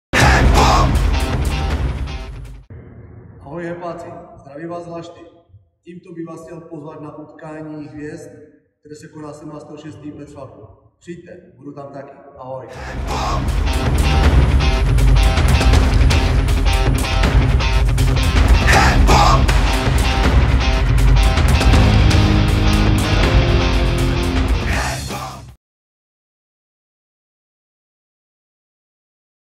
90 TJ Hepo Petřvald 17.6. 2023 - Jan Laštůvka
Vážení přátelé, další hvězdu zápasu osobností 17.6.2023 na TJ HEPO není potřeba dlouze představovat. Jedním slovem:
Lašty 🥳 Pro ty, co by snad neznali, malé info: Jan Laštůvka je český profesionální fotbalový brankář, který chytá za český klub FC Baník Ostrava. Mezi lety 2011 a 2013 odchytal také 3 utkání v dresu české reprezentace. Má za sebou působení v anglickém Fulhamu, německé Bochumi, ukrajinském Šachtaru Doněck nebo FK Dnipro. Na tuzemské scéně pusobil mimo Baník Ostrava také ve Slavii Praha nebo MFK Karviná.